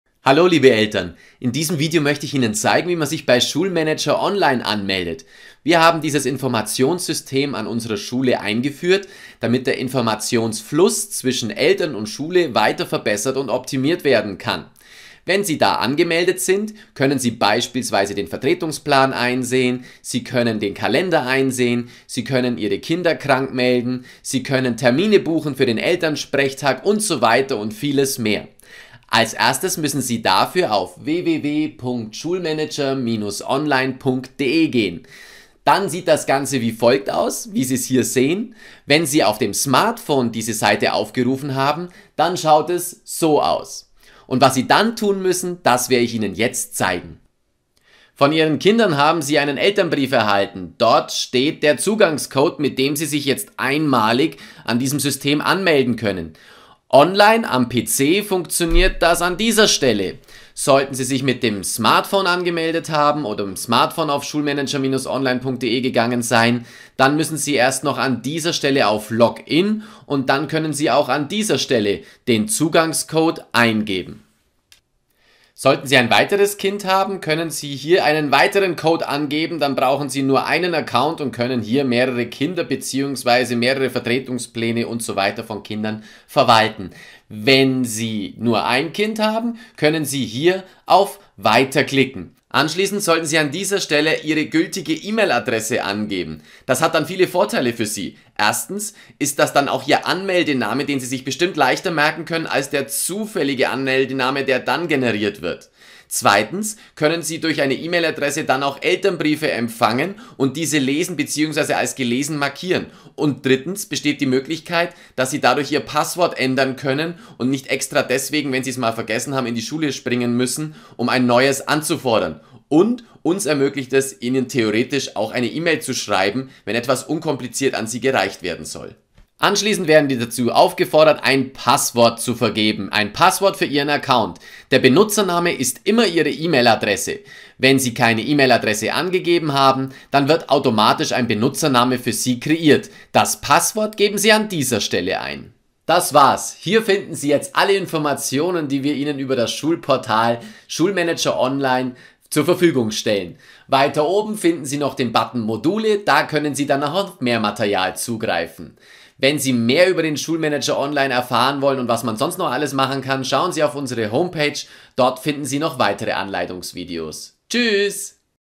Anmeldung Schulmanager-online
In diesem Video geht es um den Schulmanager, den wir für unsere Schule gebucht haben.